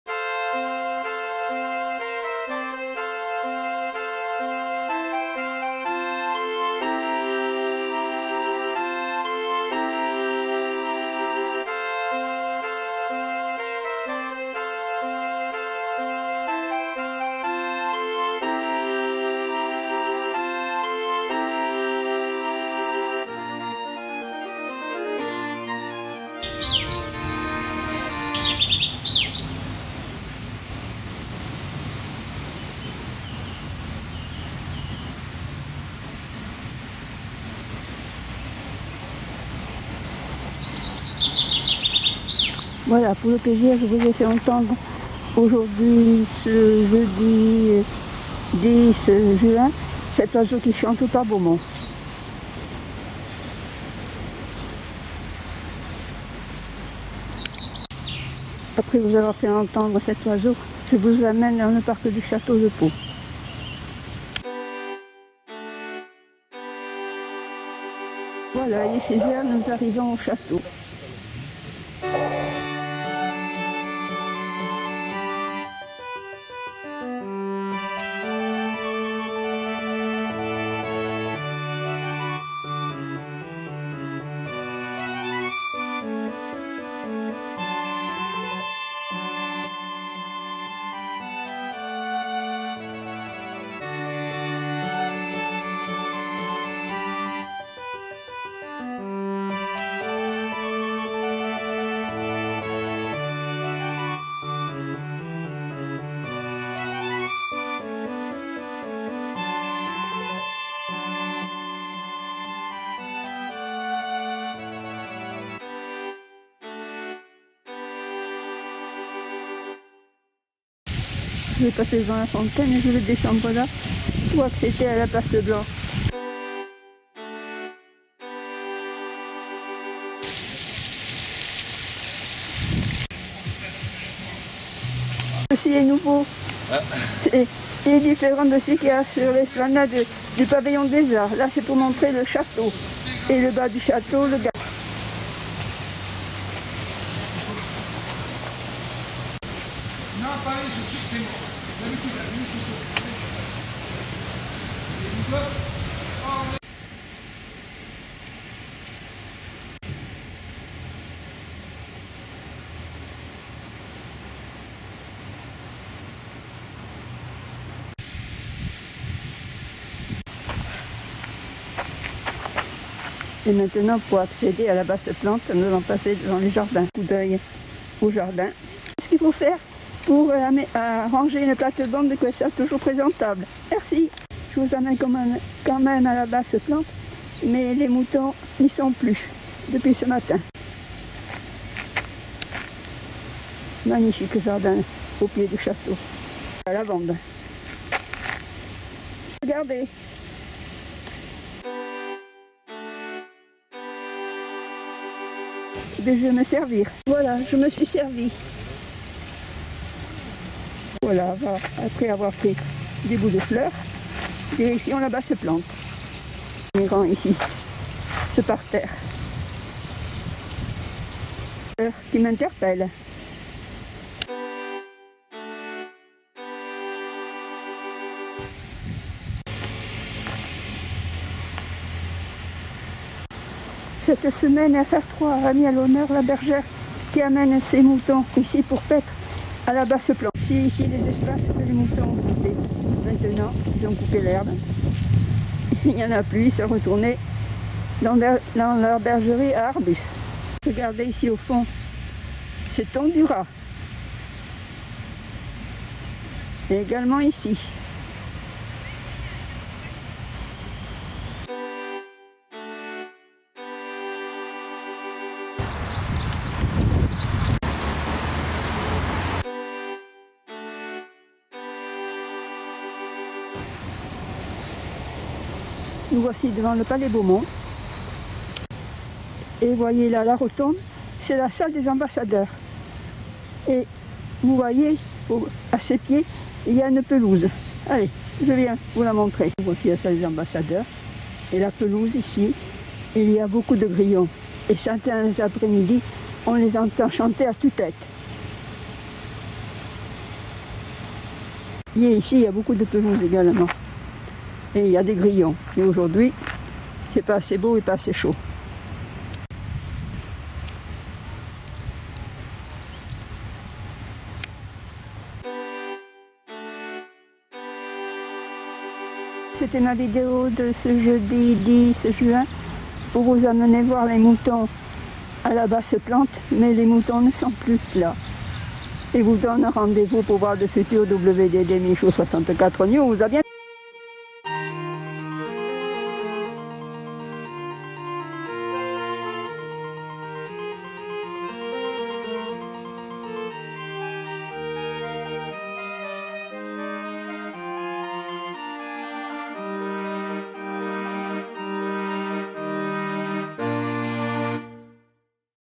LES W D D MICHOU64 NEWS 10 JUIN 2021 PAU À LA RECHERCHE DES MOUTONS DE LA BASSE PLANTE
Bonjour,
     FR3 Pau sud Aquitaine a mis à l'honneur  cette semaine avec un reportage la Bergère qui amène son troupeau de moutons pour brouter l'herbe des pelouses de la Basse Plante, ce jeudi je voulais l'interviewer et vous montrer son troupeau, hélas elle a ramené ce matin ses moutons dans leur bergerie.
     Vous souhaite de faire une bonne promenade et de passer un très bon vendredi.